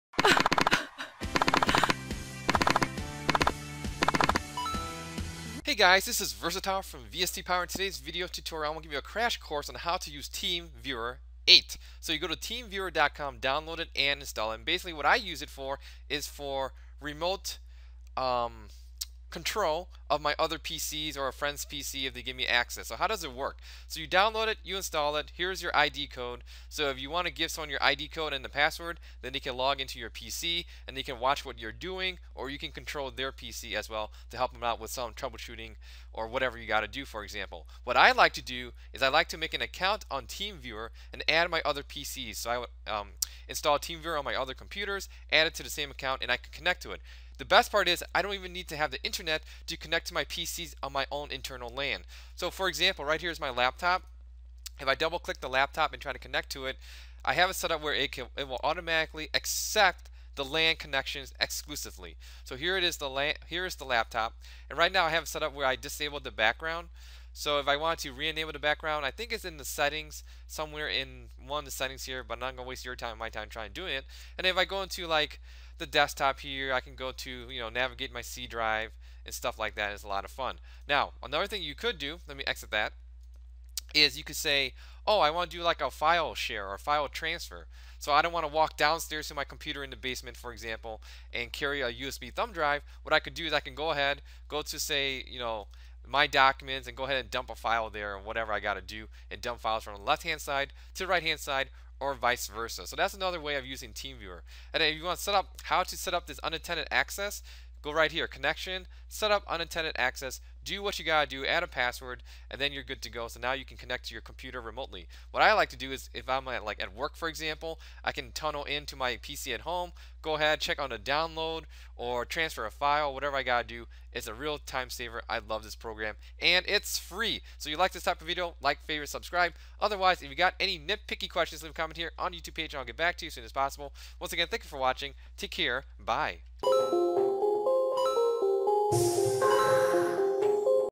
[How To] Use TeamViewer 8 For Remote Login and File Transfer Tutorial
I love TeamViewer.
Free? Yes.
Remote login without password? Yes.
File transfer? Yes.
Works without Internet? Yes.

And you can do so much more! Watch the tutorial for a crash course in how you too can be a boss right now. - Versatile